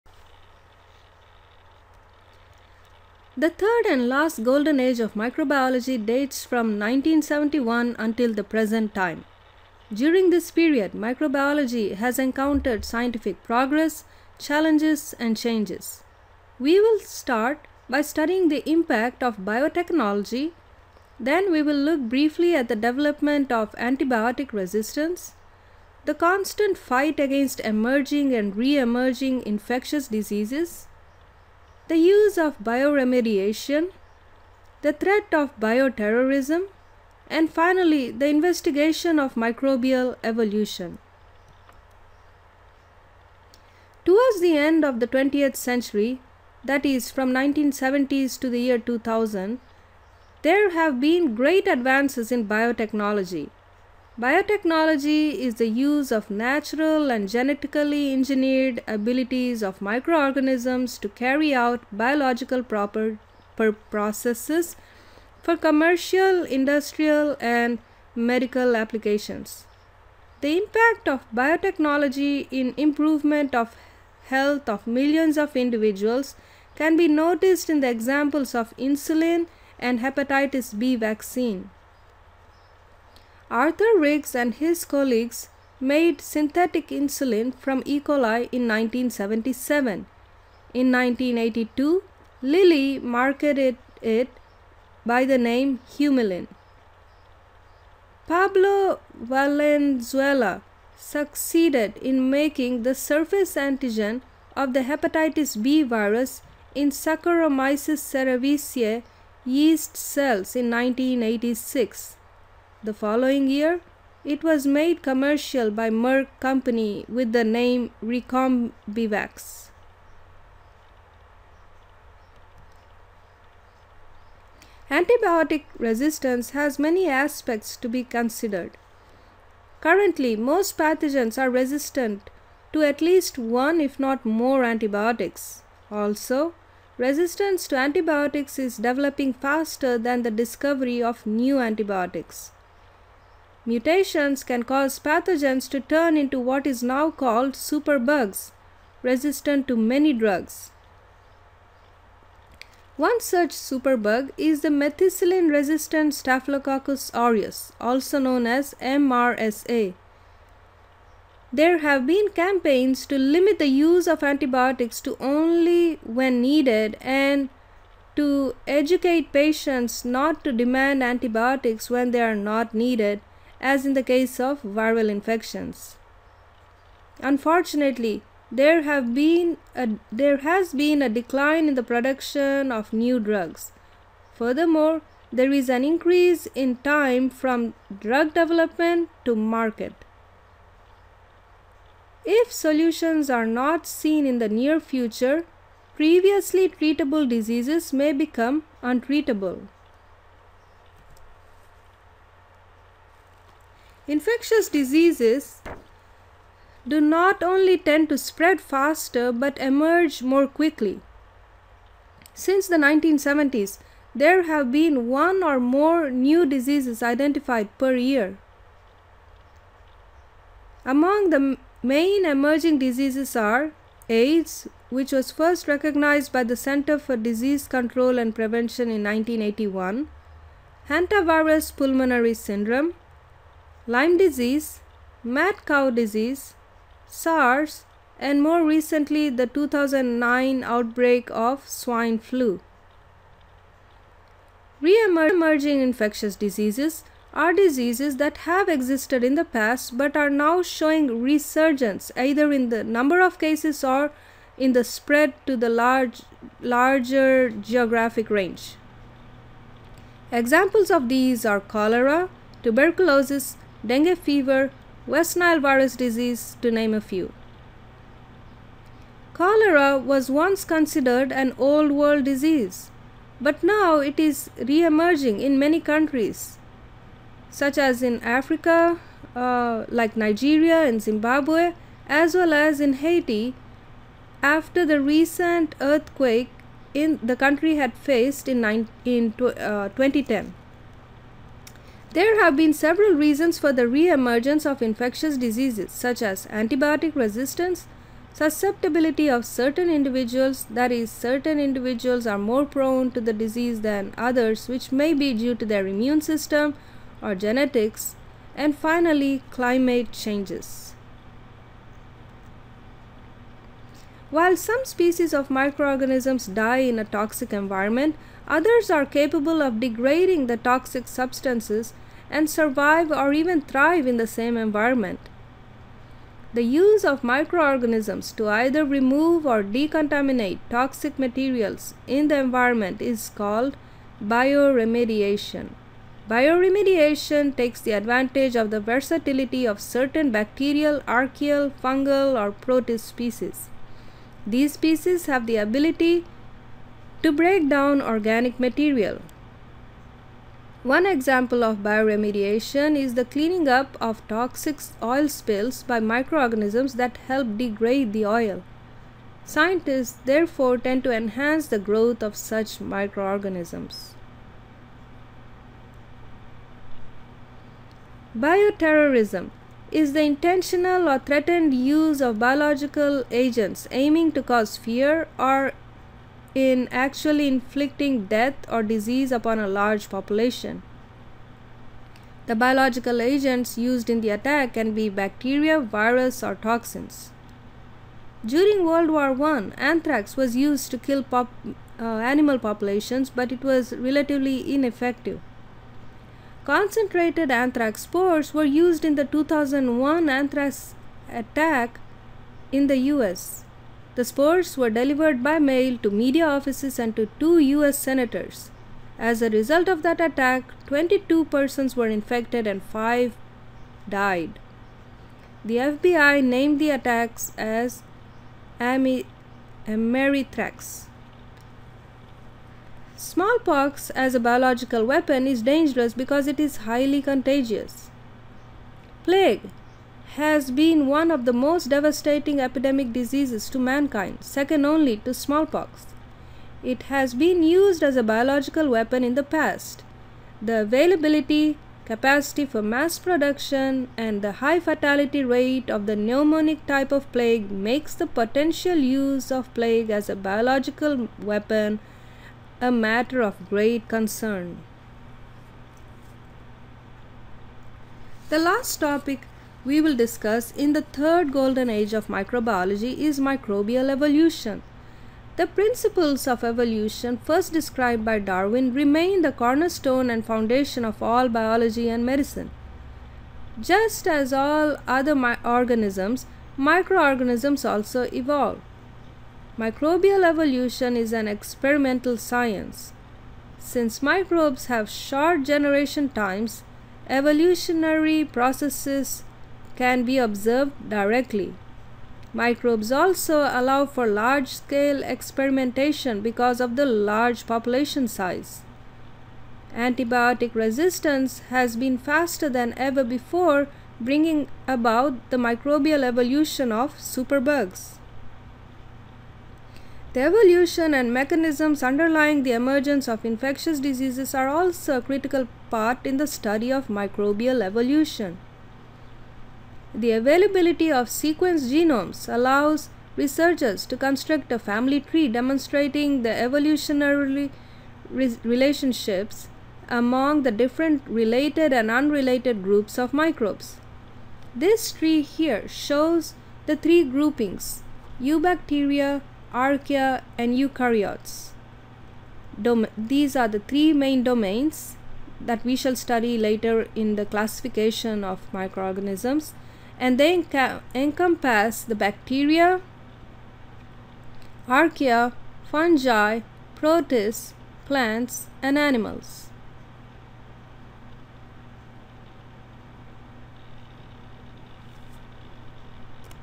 7- The Third Golden Age of Microbiology
This video describes mainly the Age we now live in, with advances in the use of biotechnology and bioremediation. At the same time, it is the era of challenges with Bioterrorism and Antibiotic resistance. Interestingly, microbial evolution, emerging and re-emerging diseases  is also noted in our era.

For more videos of the full course, including quizzes and certification of completion, please visit: